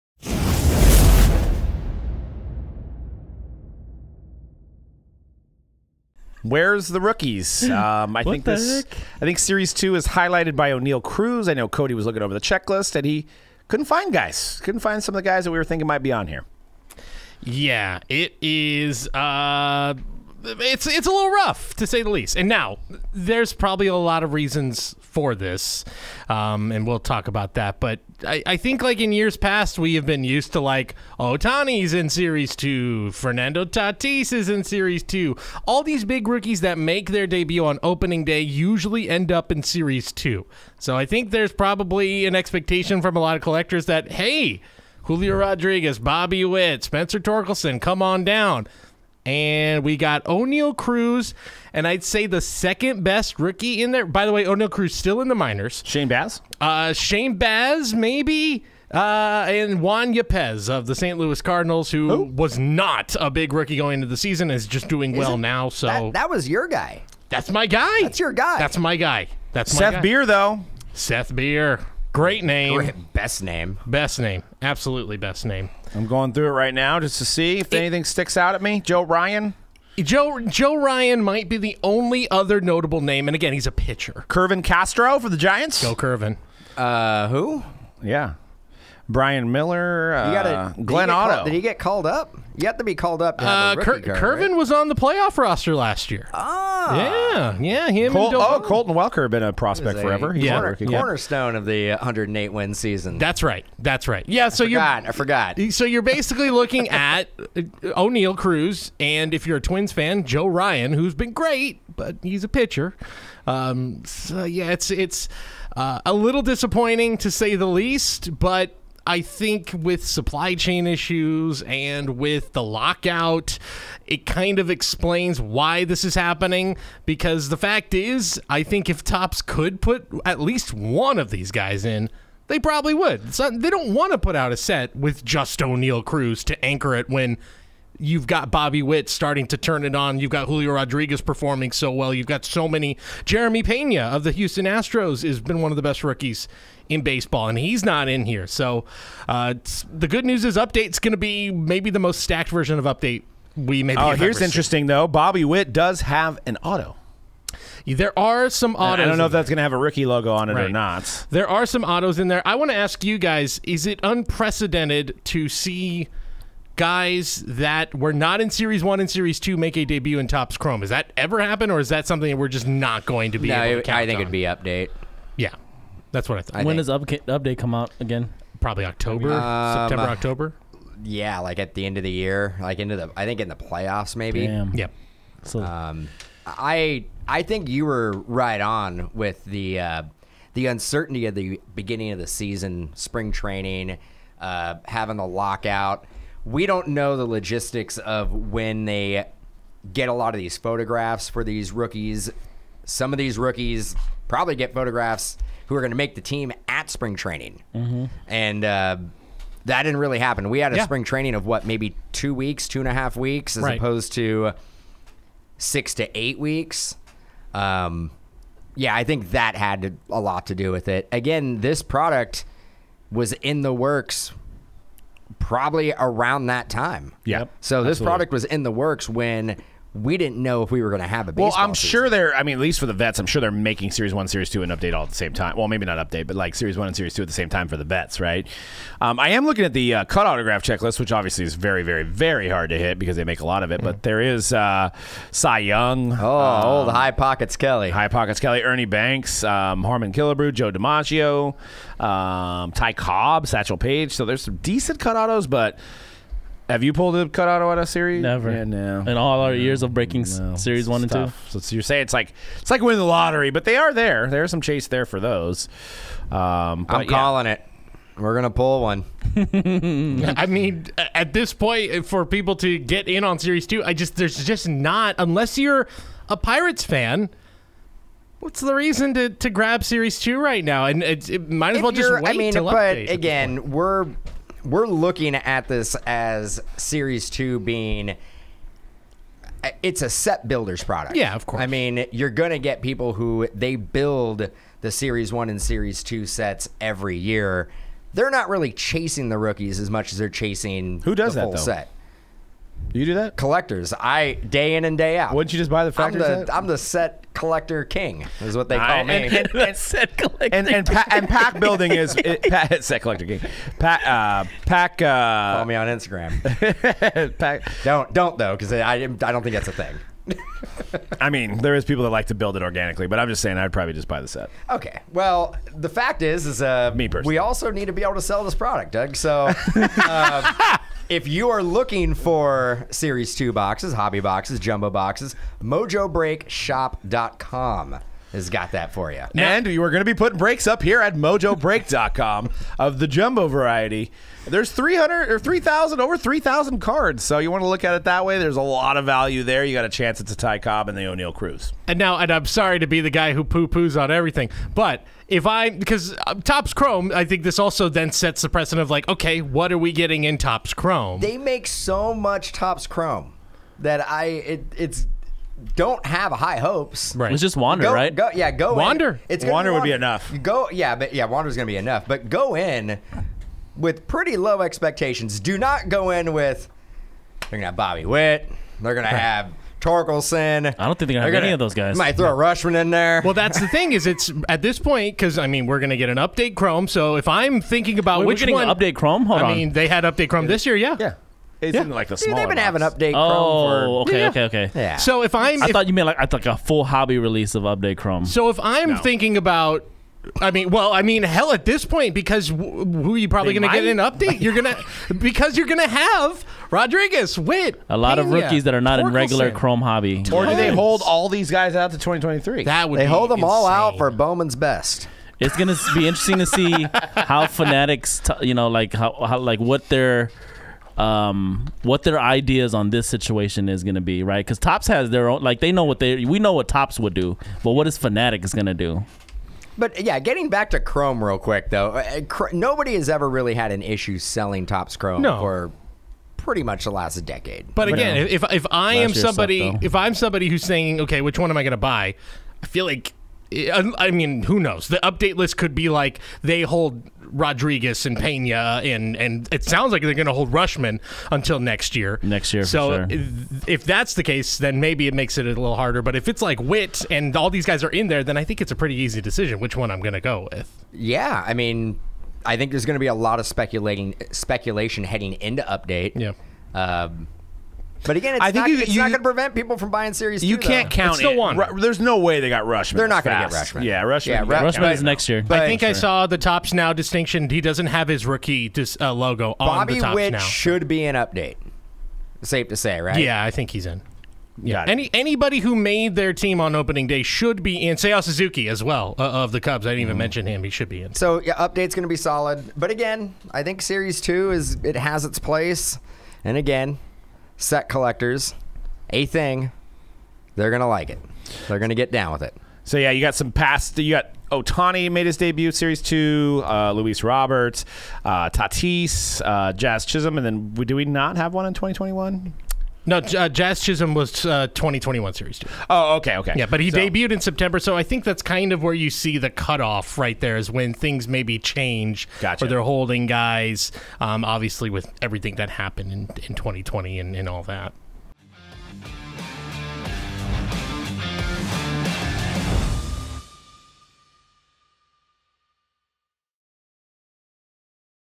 Topps Series 2 Baseball Preview | Where Are The Big Rookies?
From The Hype 242 - Topps released the checklist for the upcoming release of Series 2 Baseball and collectors were all left asking the same question; WHERE ARE THE STAR ROOKIES?? The guys address why some star rookies are missing and when we'll see their 1st rookie cards appear.